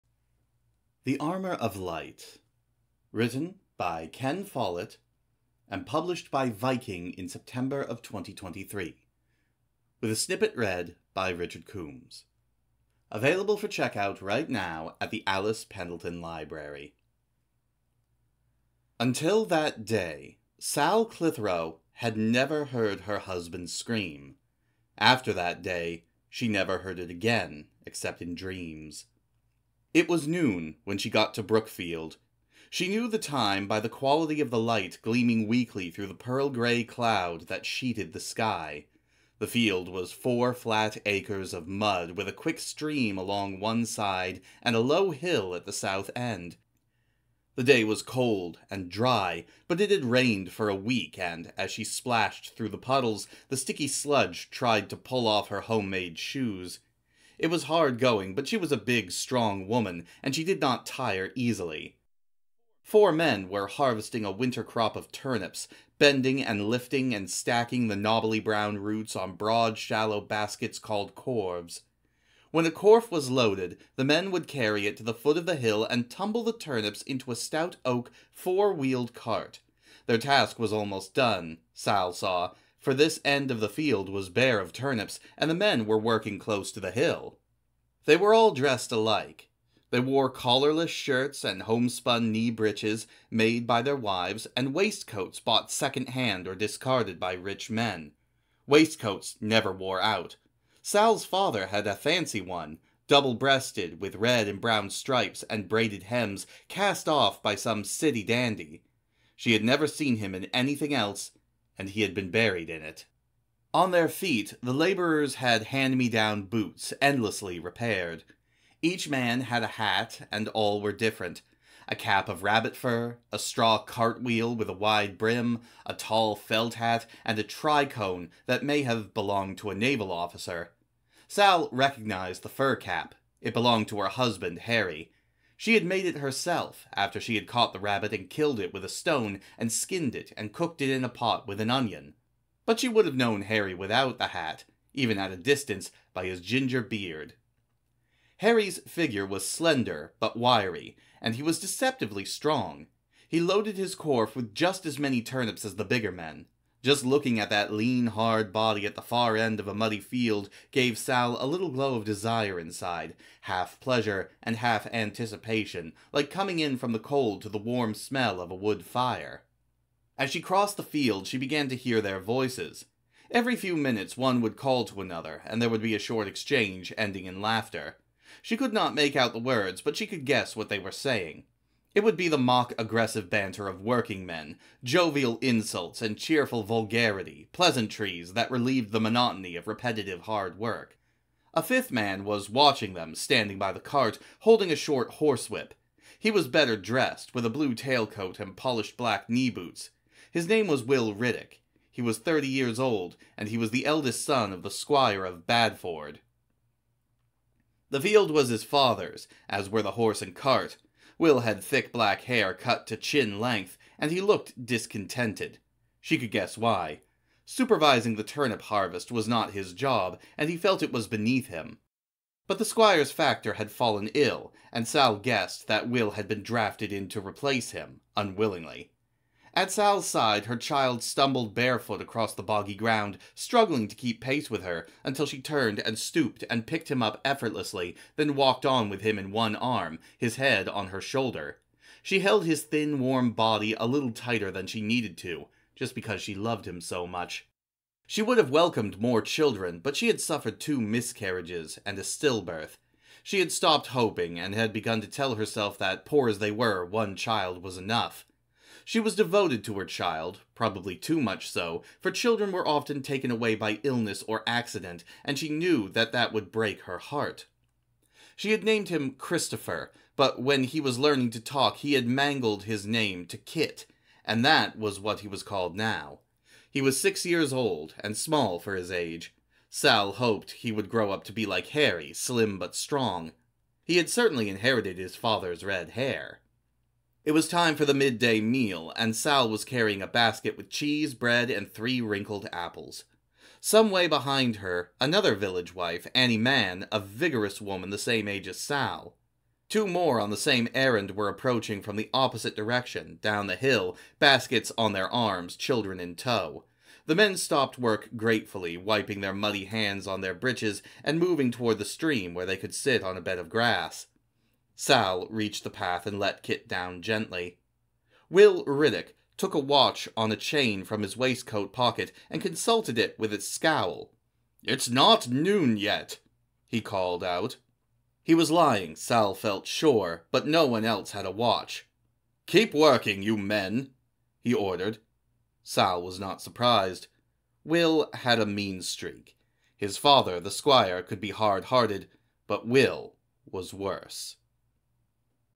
Readblog 12/16/23: "The Armor of Light" by Ken Follett
"The Armor of Light"

Written by Ken Follett

Published by Viking in September of 2023

Snippet read by Richard Coombs

"The Spinning Jenny was invented in 1770, and with that, a new era of manufacturing and industry changed lives everywhere within a generation. A world filled with unrest wrestles for control over this new world order: A mother’s husband is killed in a work accident due to negligence; a young woman fights to fund her school for impoverished children; a well-intentioned young man unexpectedly inherits a failing business; one man ruthlessly protects his wealth no matter the cost, all the while war cries are heard from France, as Napoleon sets forth a violent master plan to become emperor of the world. As institutions are challenged and toppled in unprecedented fashion, ripples of change ricochet through our characters’ lives as they are left to reckon with the future and a world they must rebuild from the ashes of war.

Over thirty years ago, Ken Follett published his most popular novel, The Pillars of the Earth. Now, with this electrifying addition to the Kingsbridge series we are plunged into the battlefield between compassion and greed, love and hate, progress and tradition. It is through each character that we are given a new perspective to the seismic shifts that shook the world in nineteenth-century Europe."